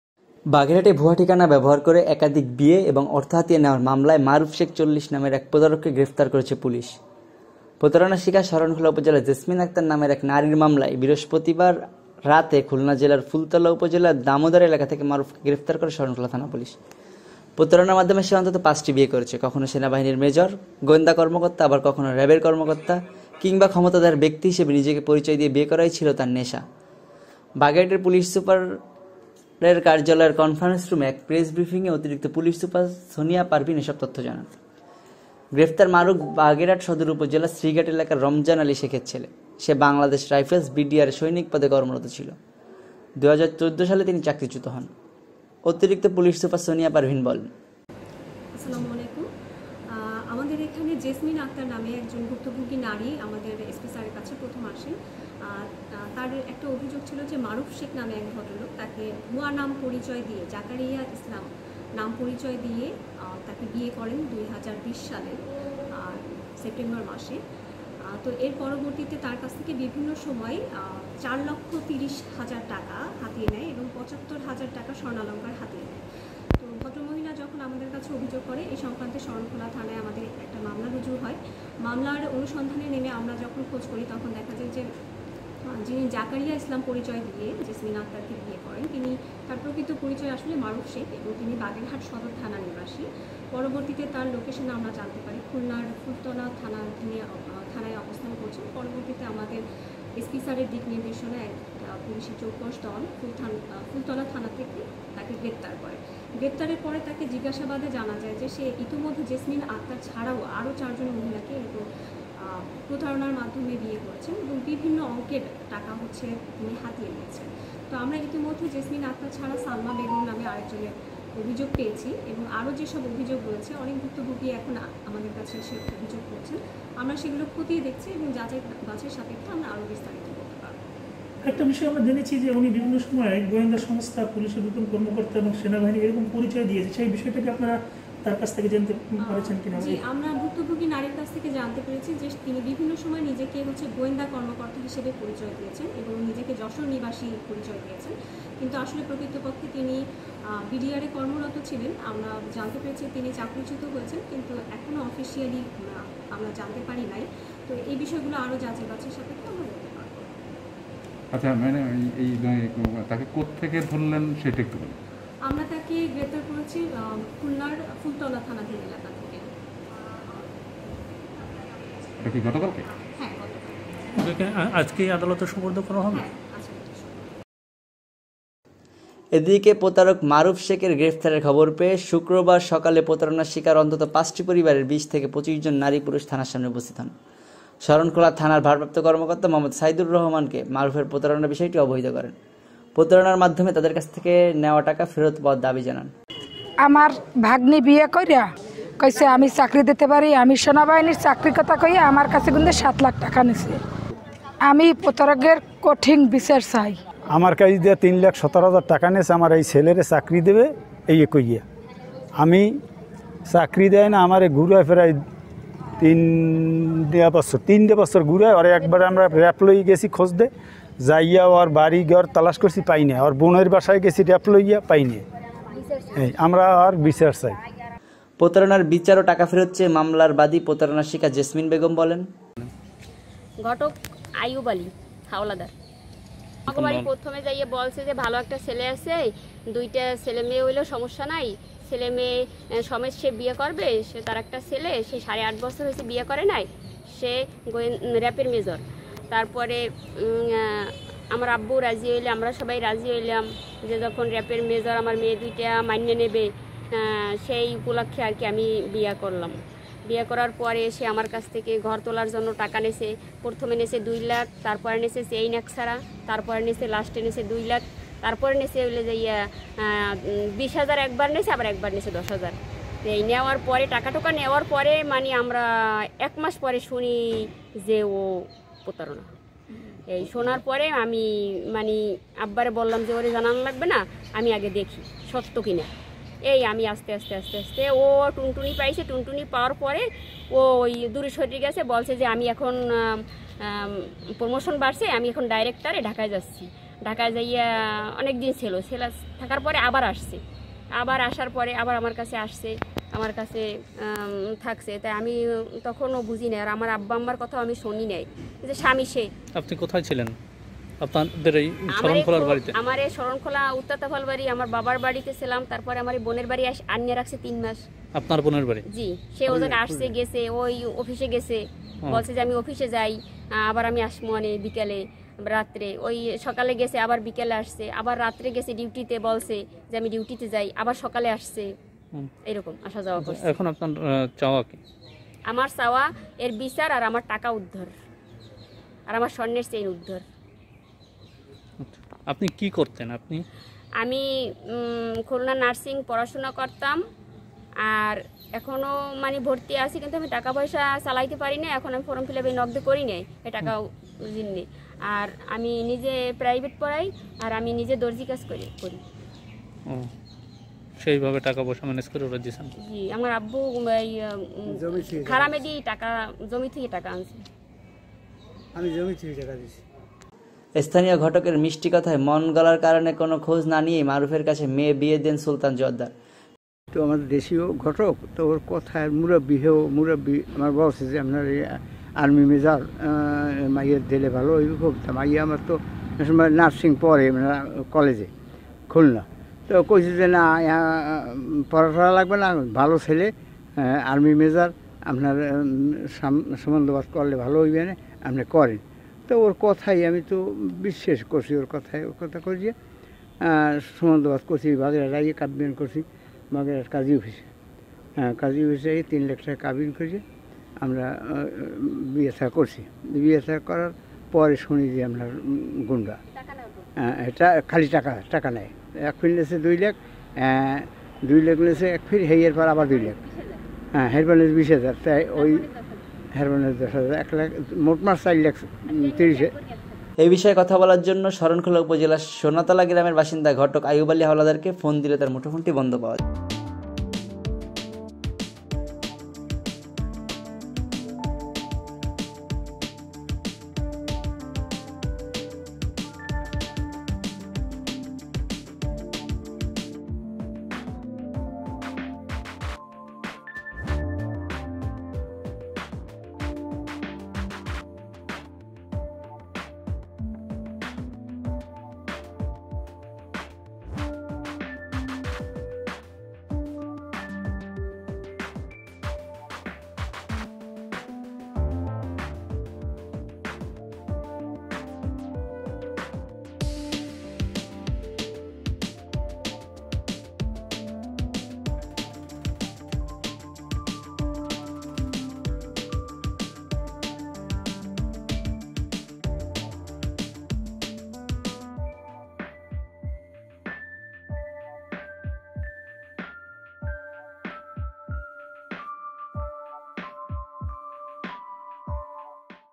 বিয়ে করাই যার নেশা, অবশেষে খেলেন ধরা
কখনও সেনাবাহিনীর মেজর, গোয়েন্দা কর্মকর্তা, আবার কখনও র‌্যাবের কর্মকর্তা কিংবা ক্ষমতাধর ব্যক্তি নিজেকে হিসেবে পরিচয় দিয়ে অন্তত পাঁচটি বিয়ে করেছেন